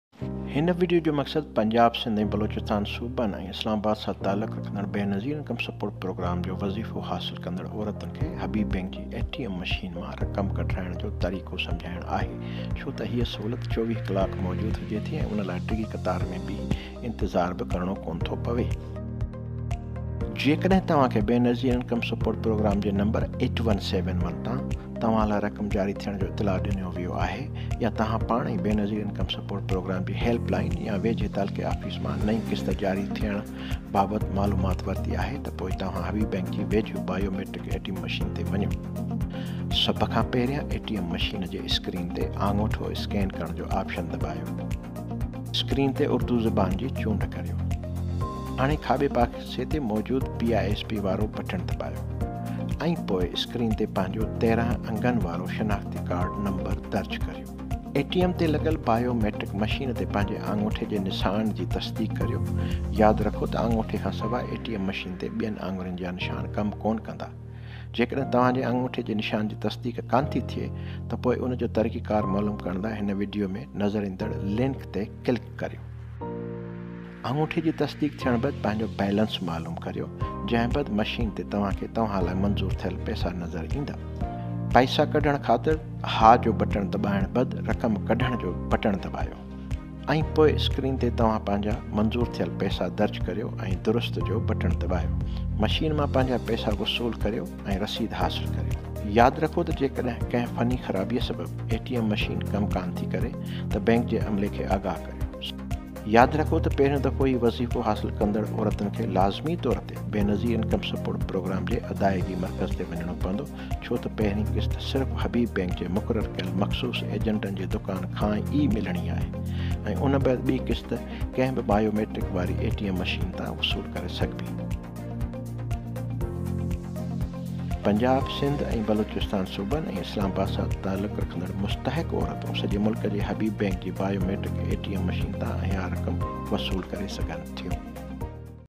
ویڈیوکامقصد بینظیرانکم سپورٹ پروگرام اہل خواتین کواے ٹی ایم سےرقم نکلوانےکا طریقہ سمجھاناہے(سندھی)
هن وڊيو جو مقصد پنجاب، سنڌ ء بلوچستان صوبن ء اسلام آباد سان تعلق رکندڙ بينظير انڪم سپورٽ پروگرام جو وظيفو حاصل ڪندڙ عورتن کي حبيب بينڪ جي اي ٽي ايم مشين مان رقم ڪڍرائڻ جو طريقو سمجهائڻ آهي